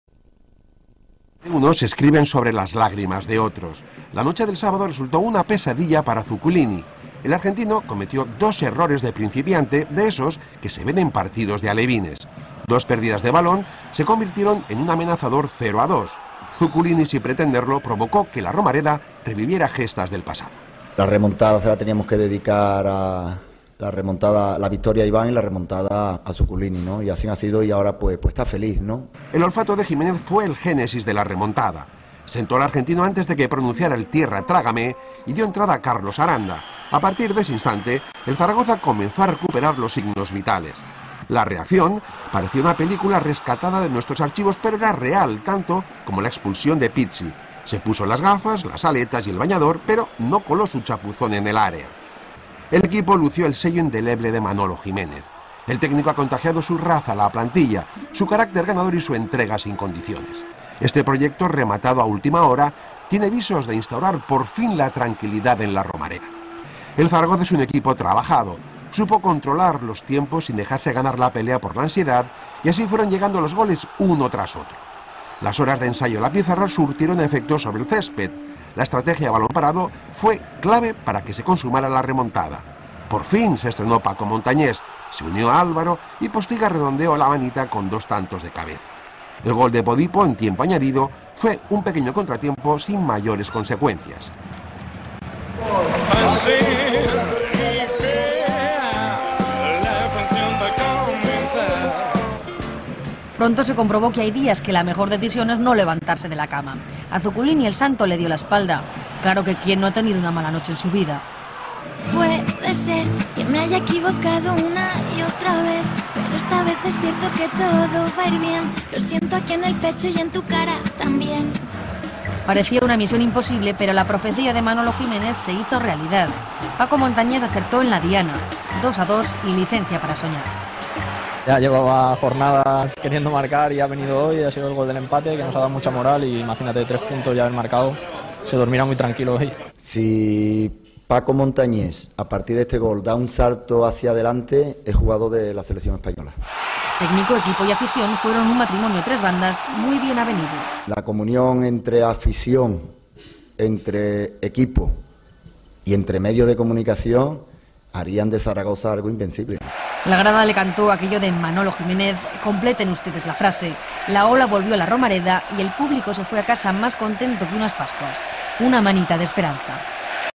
Liga 2012/13 Real Zaragoza 5-3 Deportivo
La Romareda - 11/11/2012 - Jornada 11/1ªDivisión

Real Zaragoza 5 (Apoño, Montañés, Álvaro, Postiga(2))
Deportivo 3 (Bruno, Riki, Bodipo)